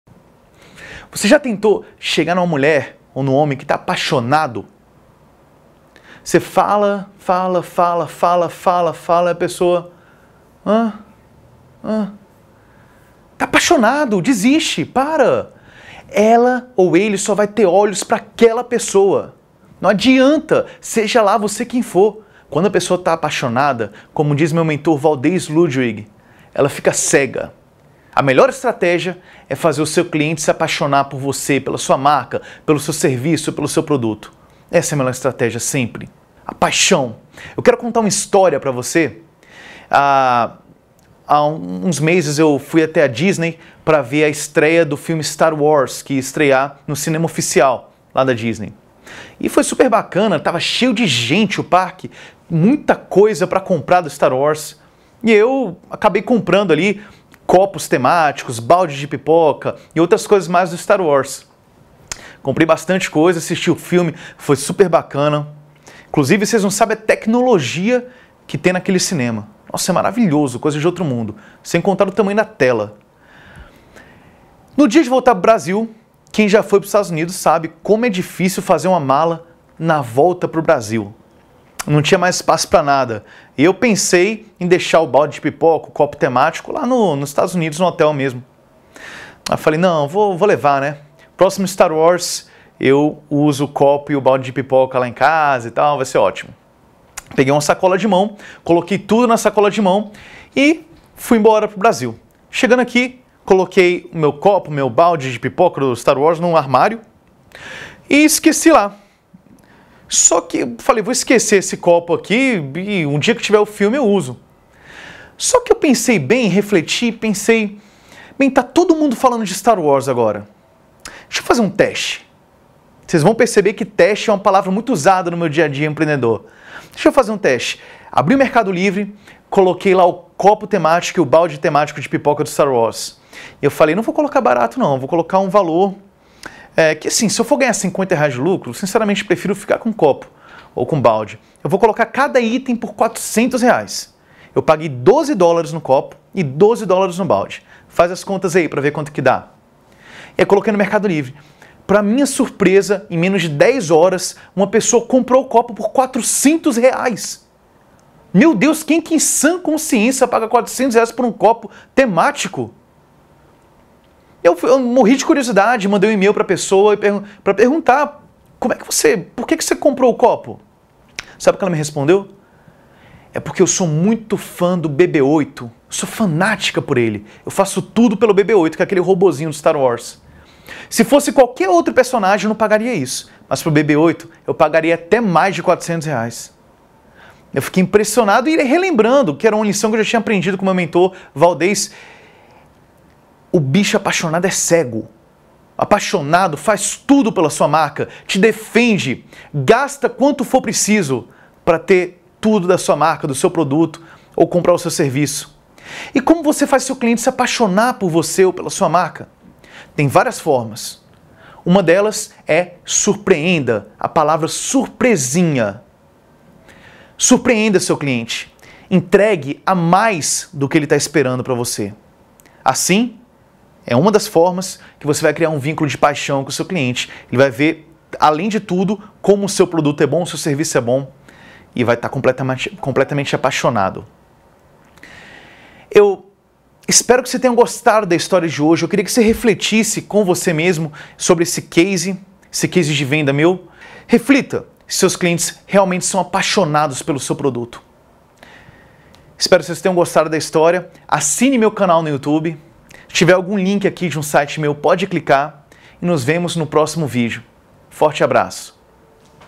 O dia em que eu vendi um copo por R$ 400,00
Entenda o Case real de vendas que eu criei, comprando um copo por 12 dólares e vendendo em menos de 24 horas por 400 reais.